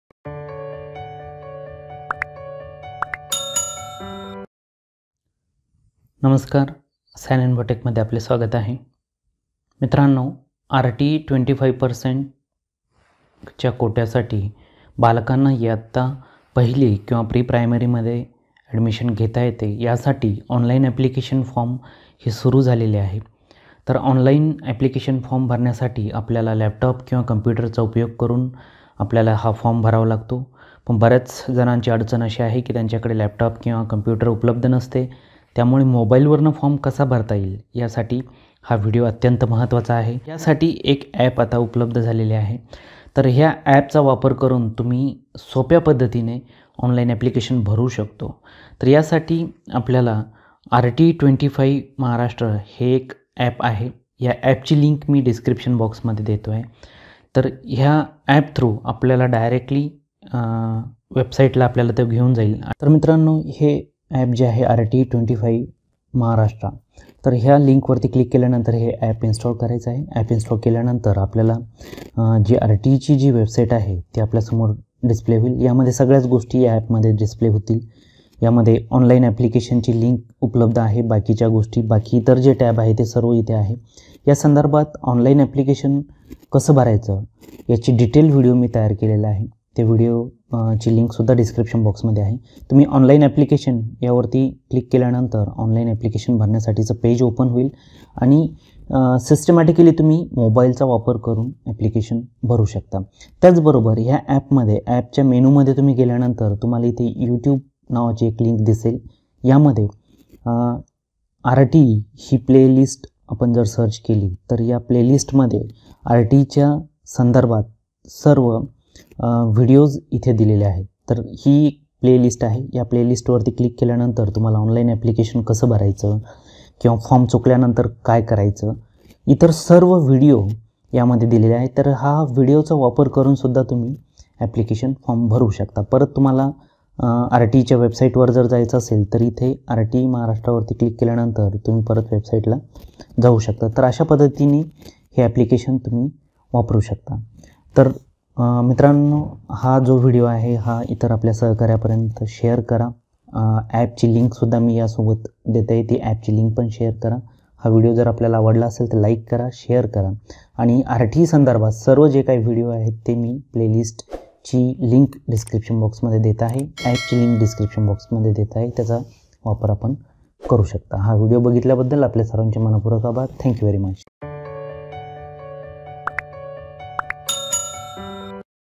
RTE 25% Maharashtra Mobile App | मोबाईल वरून RTE फोर्म कसा भरायचा?
RTE 25% प्रवेशासाठी अर्ज भरण्याची प्रक्रिया सुरु आहे. अर्ज भरण्यासाठी प्रत्येकाकडे कॉम्पुटर किवा Laptop उपलब्ध नसतो. तर अशावेळी Mobile वरून फोर्म भरण्यासाठी एक App उपलब्ध केले आहे. या App चा वापर करून सोप्या पद्धतीने RTE 25% ची वेबसाईट हाताळता येईल.

Thank you viewers and YouTube!